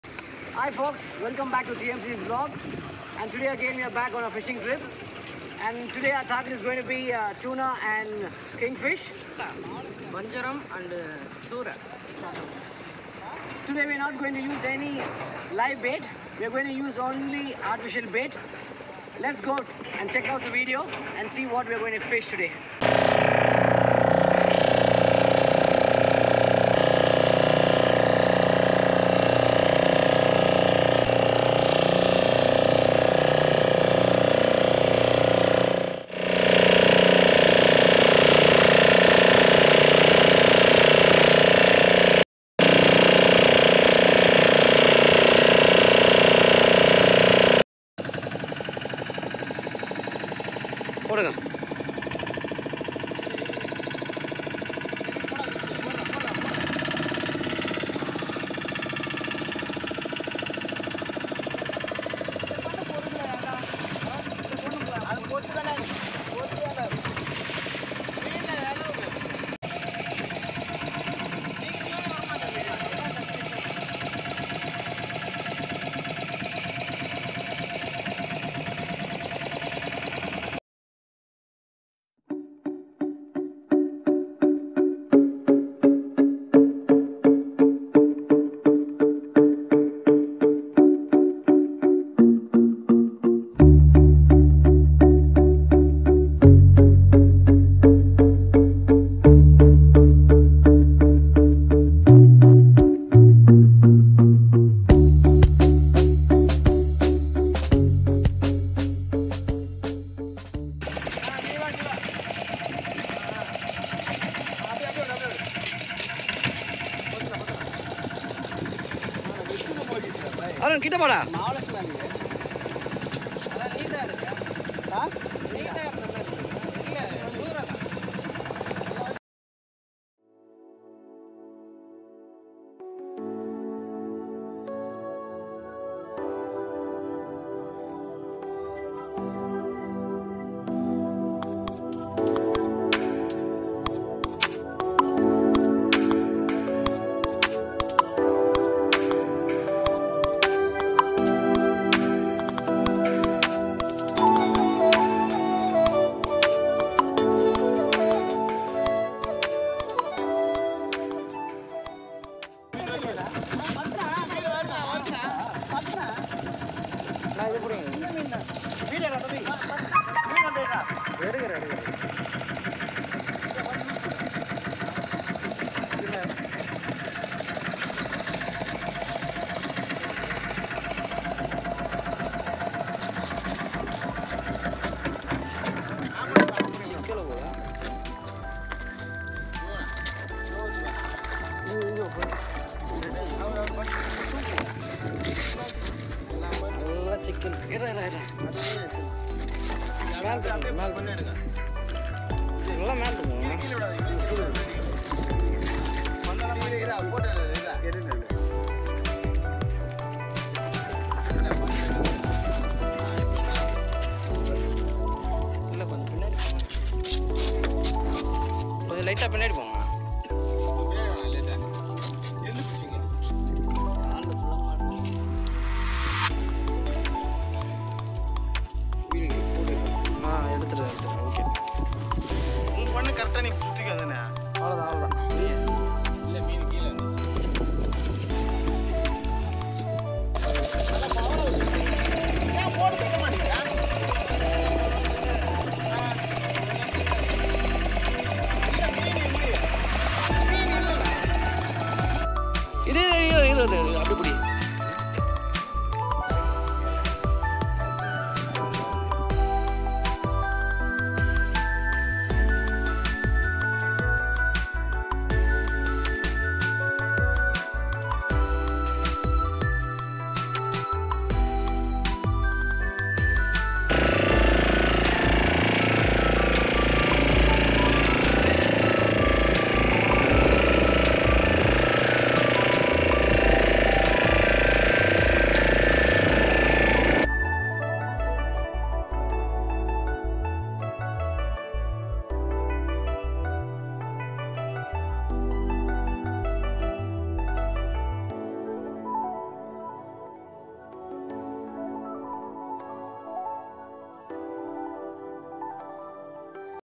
KingFish Caught in Chennai#Big Fish Caught# TMC Vlogs# Tuna fish caught back to back..
Hello to all my Fishing lovers,

Welcome Back!

In this video we had caught a big fish using Artificial baits. This is one of the oldest traditional to catch a big fish. Enjoy the video my dear fishing lovers.

Enjoy your fishing...

Love & Peace
TMC Vlogs